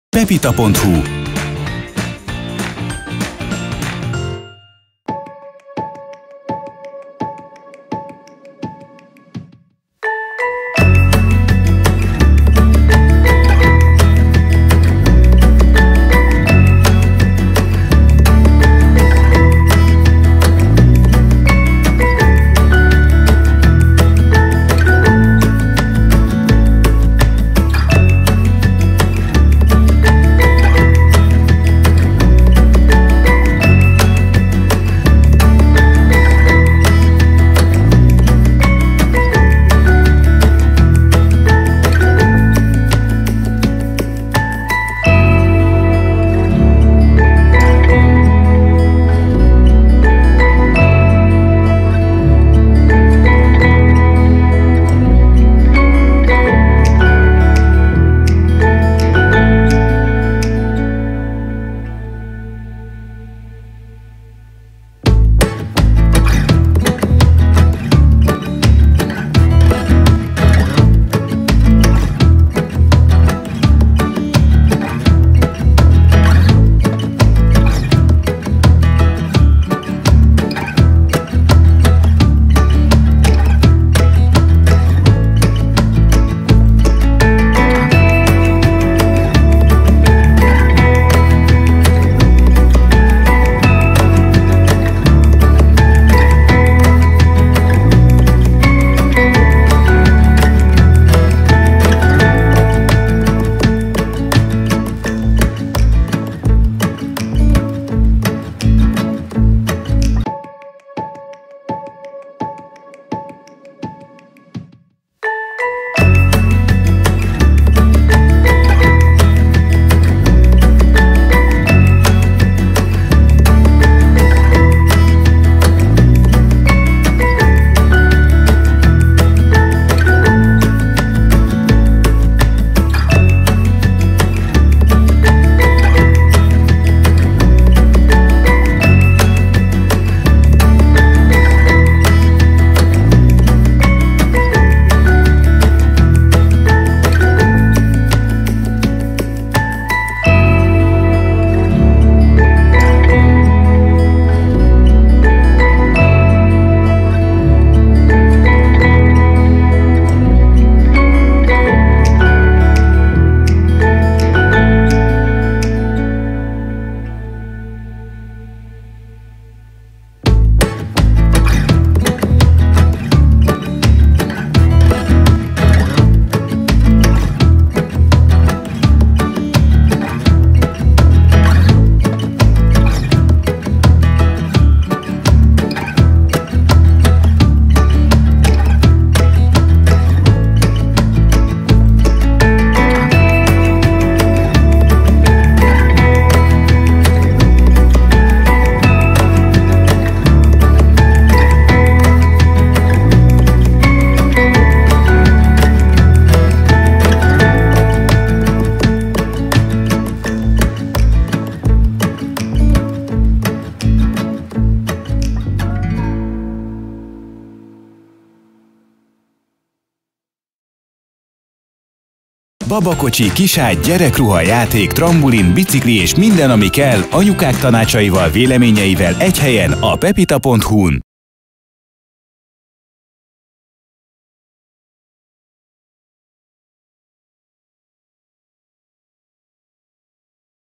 Kinderkraft Prime 3in1 multifunkciós babakocsi - Pepita.hu
A Kinderkraft Prime 3in1 multifunkciós babakocsija minden olyan  tulajdonsággal, funkcióval, kiegészítőkkel rendelkezik, amelyekre csak szükségetek lehet! Kiváló minőségű anyagok felhasználásával jött létre, ezzel biztosítva, hogy hosszú időn keresztül kiszolgál titeket. Gyönyörű, modern és egyedi megjelenésű, amely minden tekintetet magára vonz. Szép kivitelezése mellett, nagyon kényelmes, biztonságos használatot nyújt gyerkőcöd számára, és számodra is egyaránt.